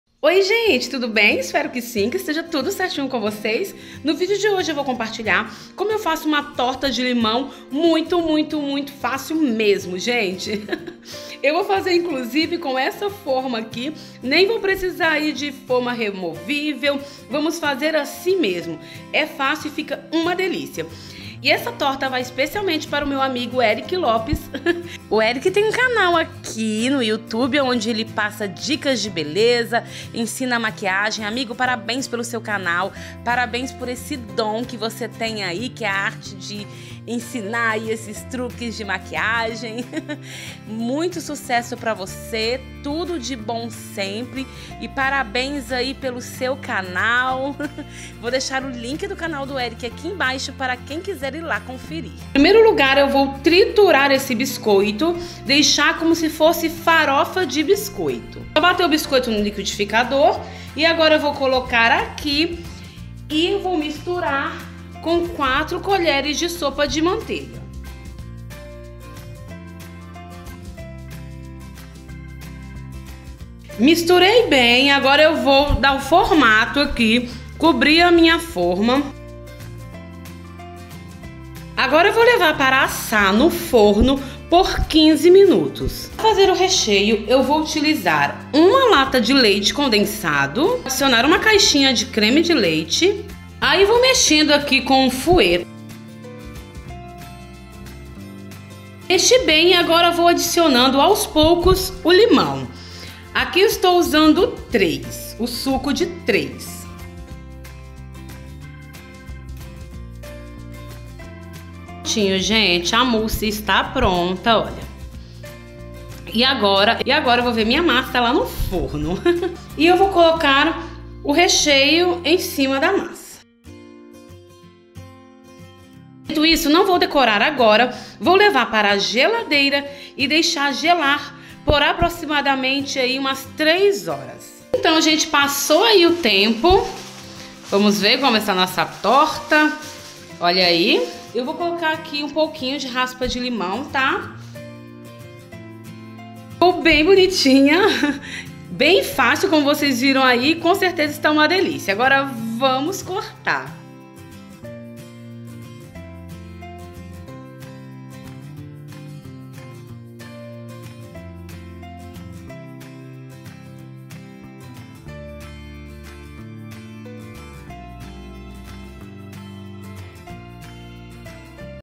TORTA DE LIMÃO USANDO BISCOITO MUITO FÁCIL + PARABÉNS MEU AMIGO ERICK LOPES
ESPERO QUE GOSTEM E SE VOCÊ GOSTOU CLIQUE NO JOINHA PARA AJUDAR NOSSA COMUNIDADE.

Ingredientes:
1 pacote de biscoito maizena;
4 colheres de sopa de manteiga;
1 lata de leite condensado;
1 lata de creme de leite;
O suco de três limões.

SUPER BEIJO NO CORAÇÃO E ATÉ O PRÓXIMO VÍDEO.